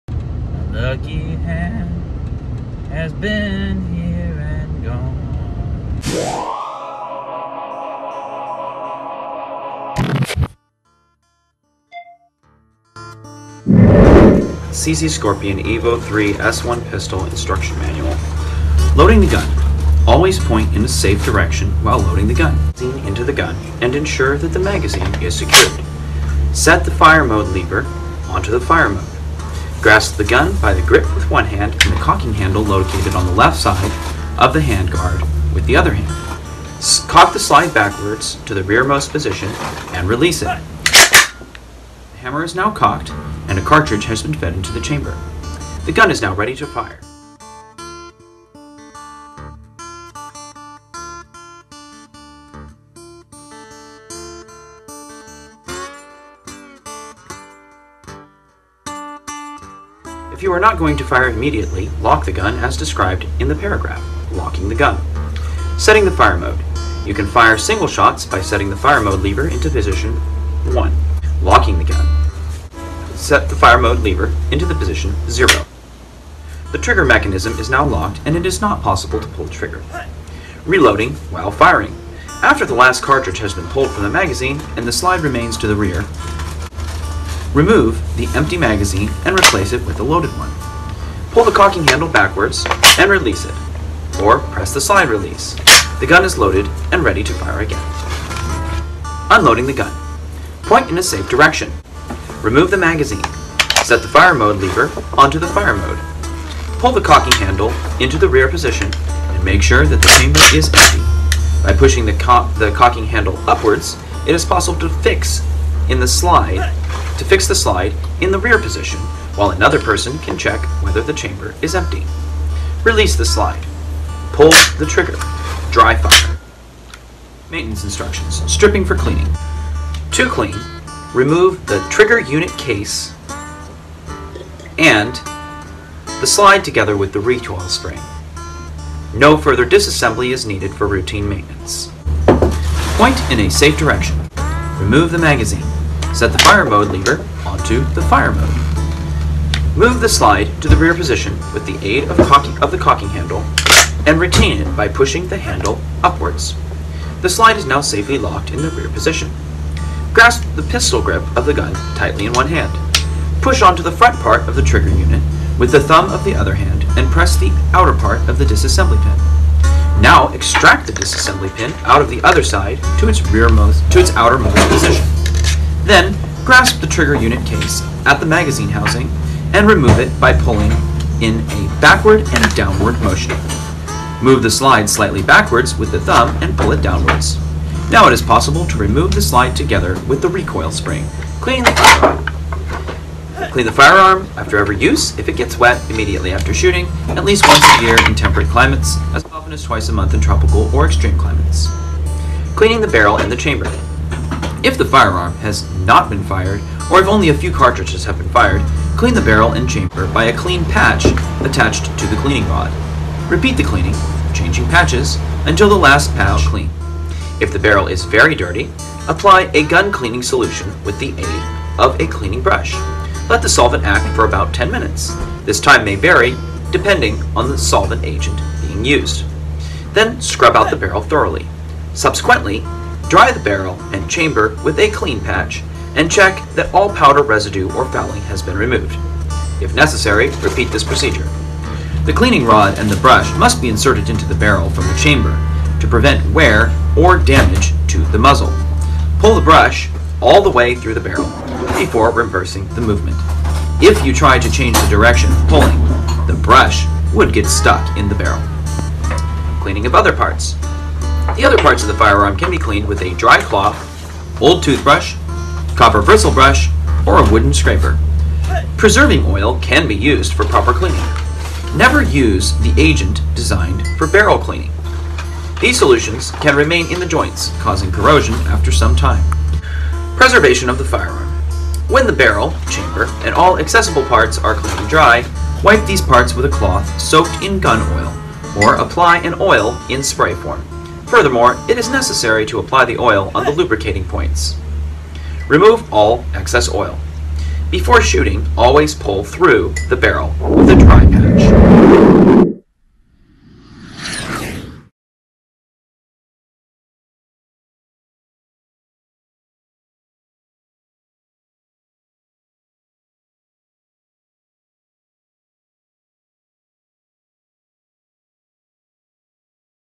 CZ SCORPION EVO 3-Firing Cleaning Lubricating-BEST Video-5min
Source: CZ SCORPION EVO 3 S1 (Civilian Semi-Auto), Manufacturer’s Instruction Manual, with demonstrations by Rusty Jem.
“The whole world is watching.”
Every Man A King